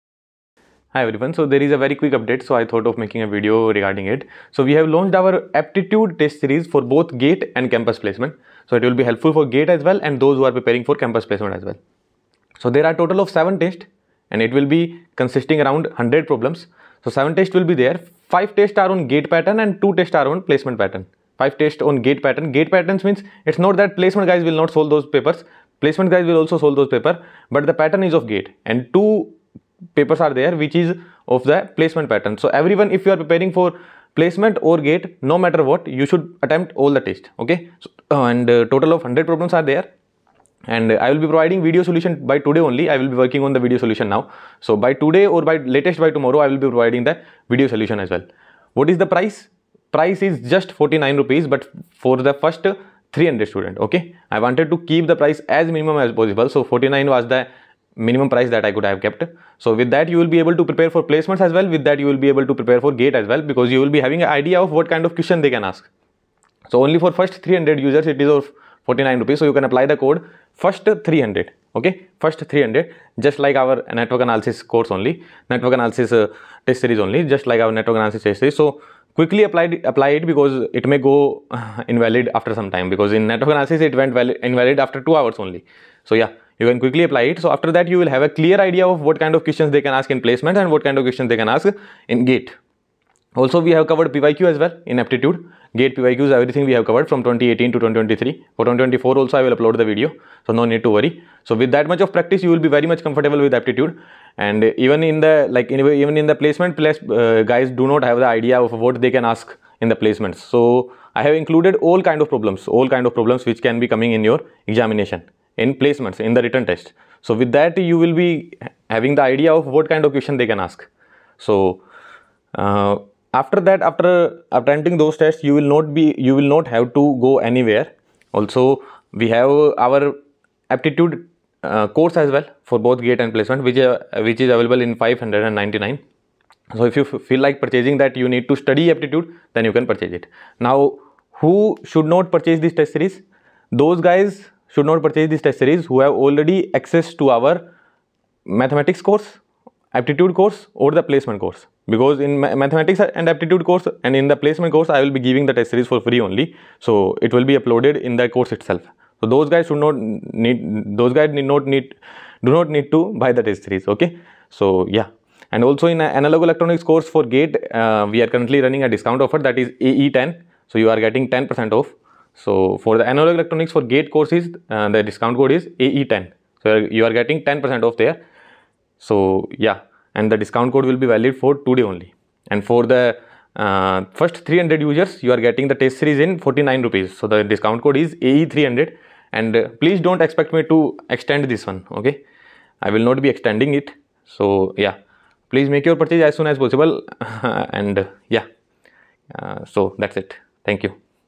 🔥Launching Test Series for Aptitude for GATE & Placements || EC/EE/IN || PrepFusion
"Analog VLSI Mastery : Cohort (0-100)" {12 Months} -

0:00 - Launching
0:33 - Features
0:35 - 1) Test Series Structure
0:55 - 2) Quality of Problems
1:16 - 3) Types of Questions
1:24 - 4) THE BEST THING
1:40 - 5) Interface
1:54 - 6) Performance Analysis
2:09 - Price?
2:29 - Promo code
2:41 - How Can You Access?
3:45 - Our Other Courses
4:24 - One more Information
5:08 - AE10